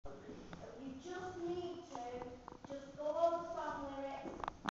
The Show's Cast - Rehearsals !
I am sat on the right side of the stage with the cast of the show we are putting on this week at our drama theatre, the director and producer are speaking to us